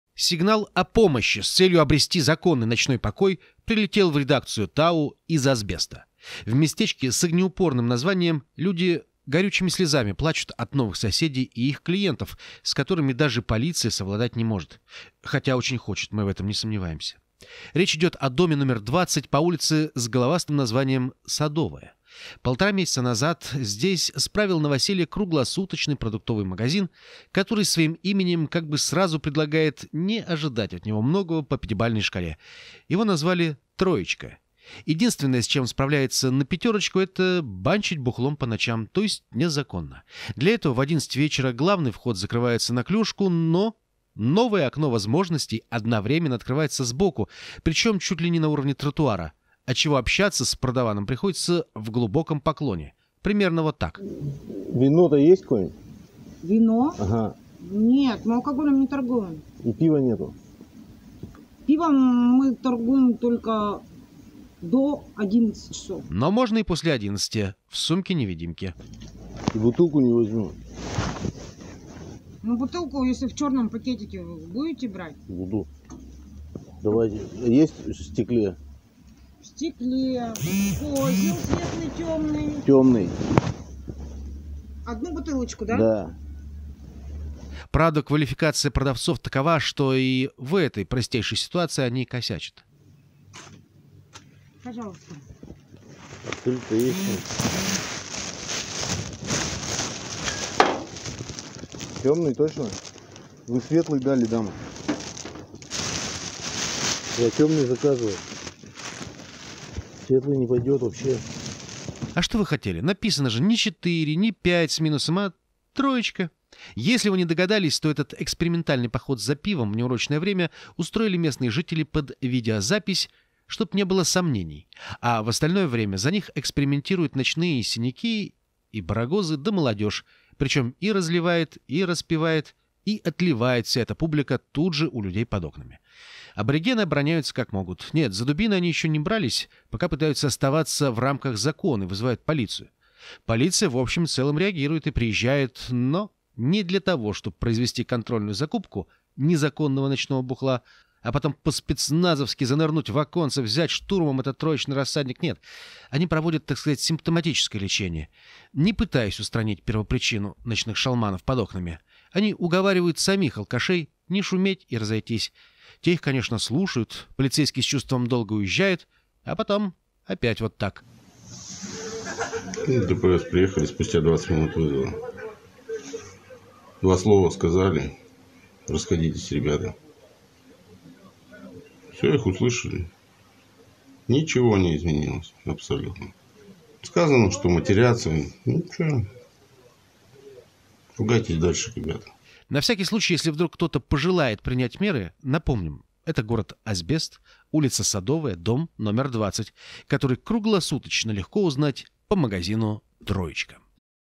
Жильцы дома взвыли от открывшегося внизу магазина.
Теперь по ночам под их окнами регулярно пьяные шалманы.